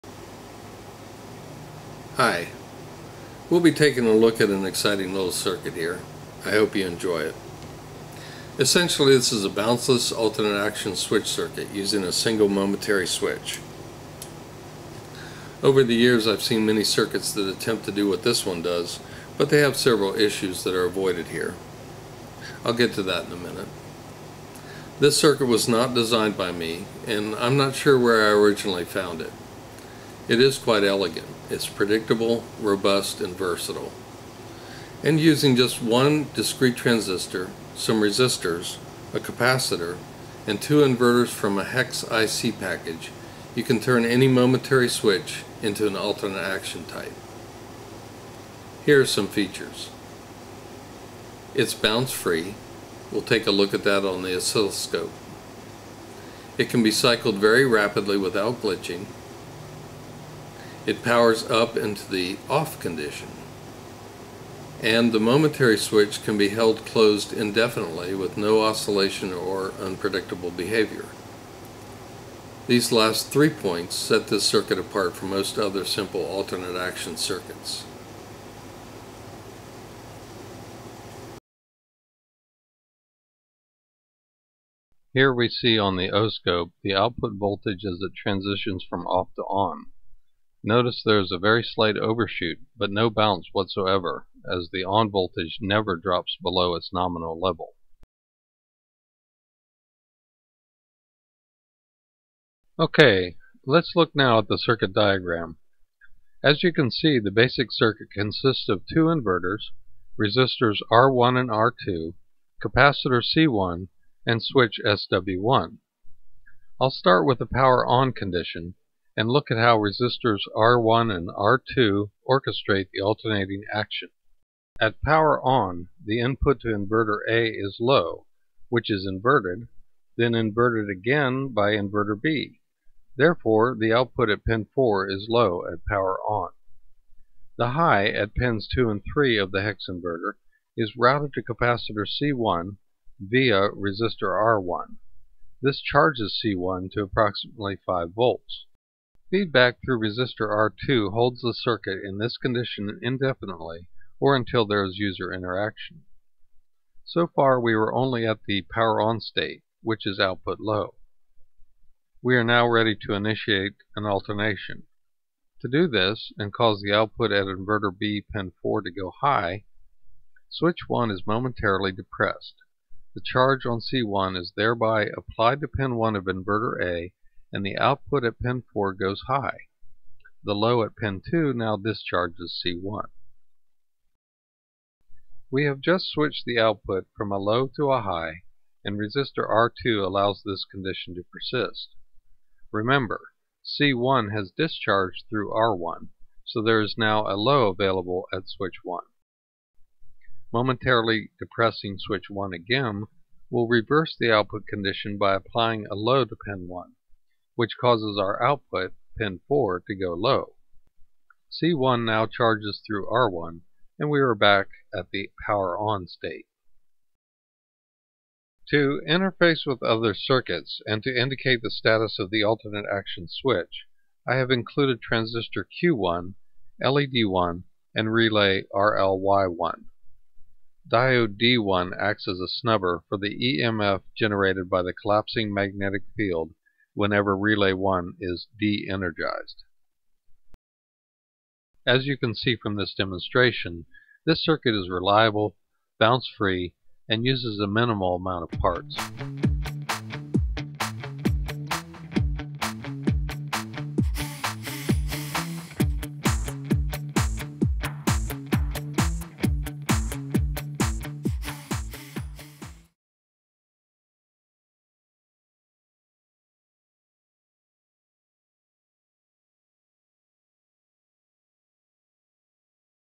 Push On - Push Off Switch Circuit Push On Push Off Alternate Action
Circuit and demonstration of a switch circuit that electronically converts a momentary pushbutton switch to a alternating on off switch.